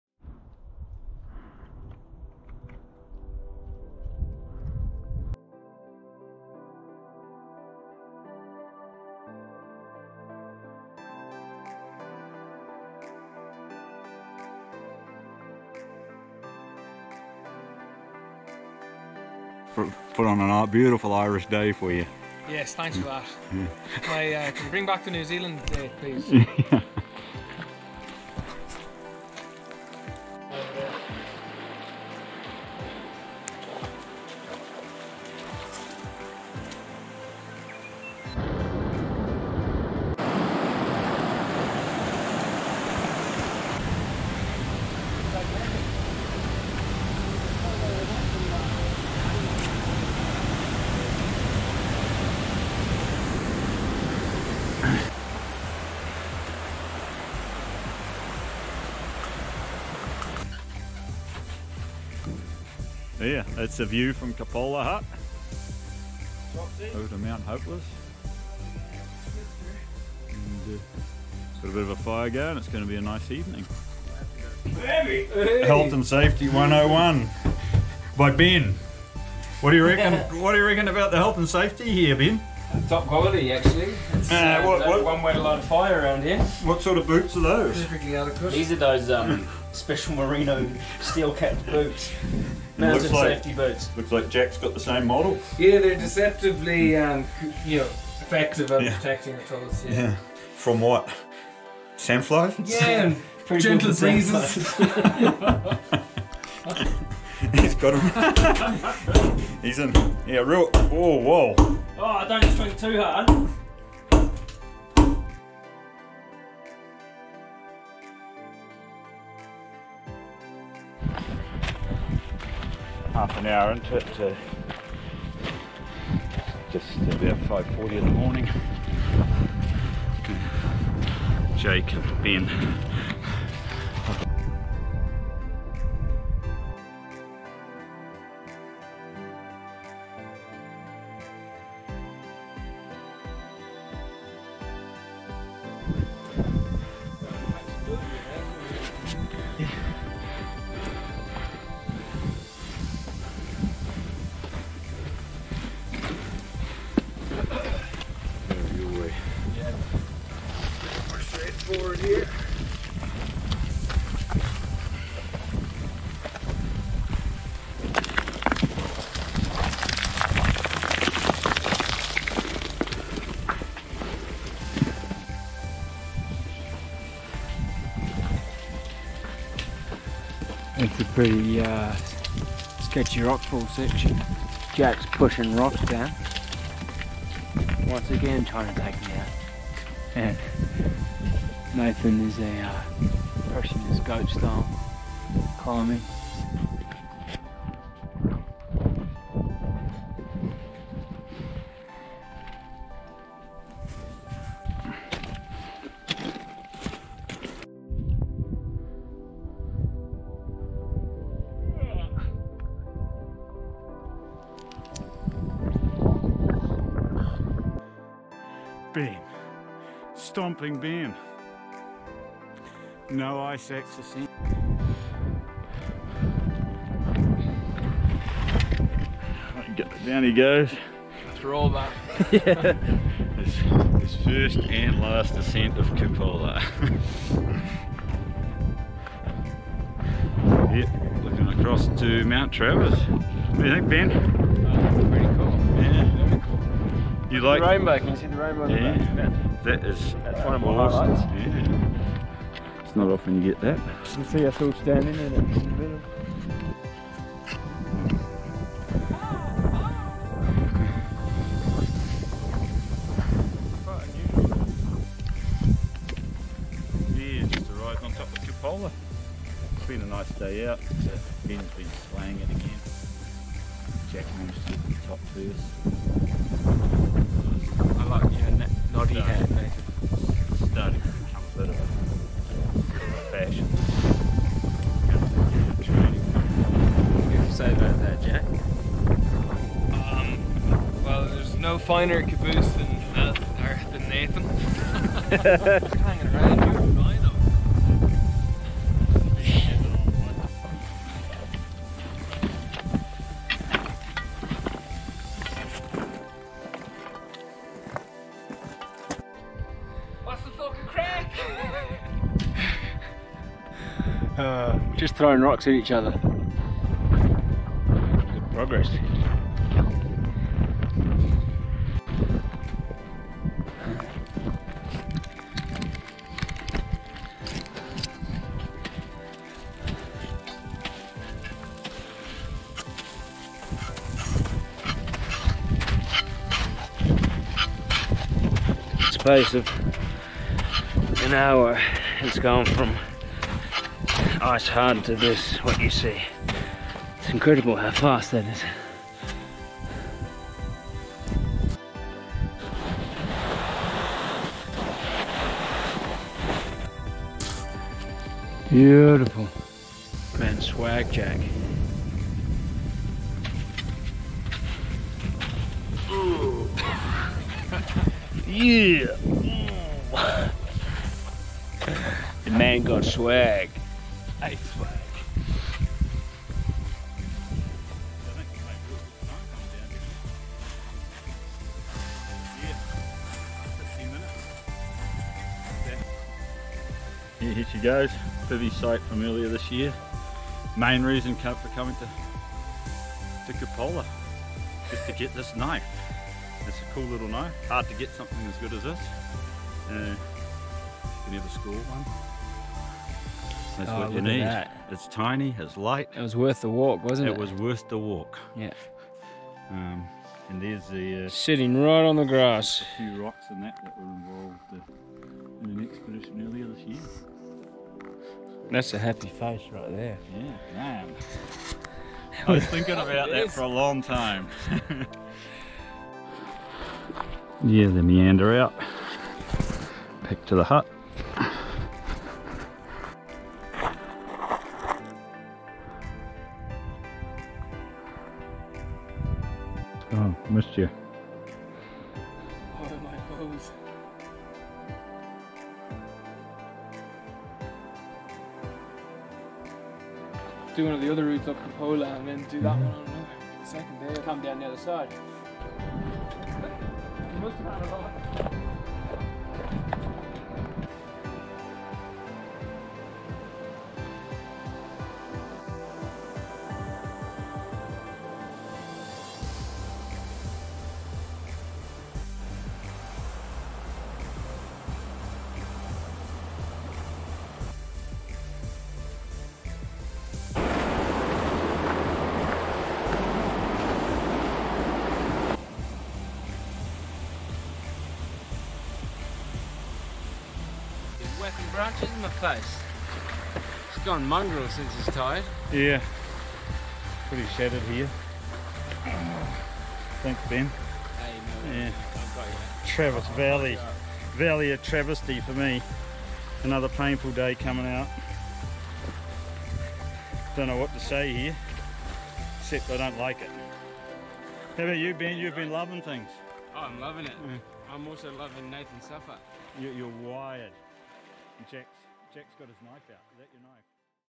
Mount Cupola
November trip to Mount Cupola in the Nelson lakes National .park